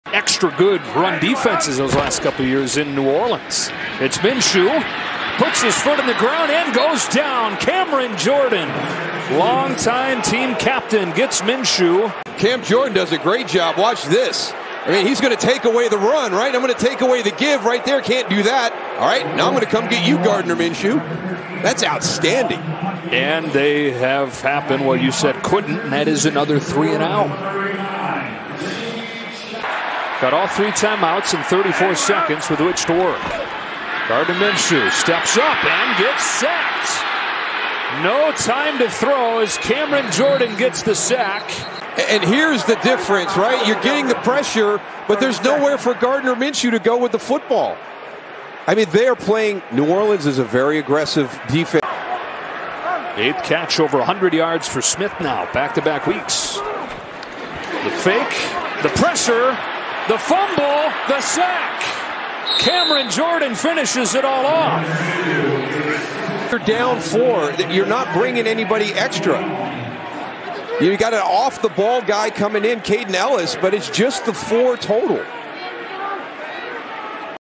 Cam Jordan Record-Setting Sacks vs Eagles | New Orleans Saints
Defensive end Cameron Jordan sacked Philadelphia quarterback Gardner Minshew three times Sunday to become the New Orleans Saints' all-time sack king with 115.5, one-half more than Pro Football Hall of Famer Rickey Jackson.

Jordan, in his 12th season out of Cal, took down Minshew twice in the first half and added the third late in the fourth quarter. He entered the Philadelphia game with 5.5 sacks on the season but quickly took advantage of Philadelphia's backup right tackle and Minshew, the backup to quarterback Jalen Hurts. It is also Jordan's 27th multi-sack game, tying him with Jackson for first place in franchise history.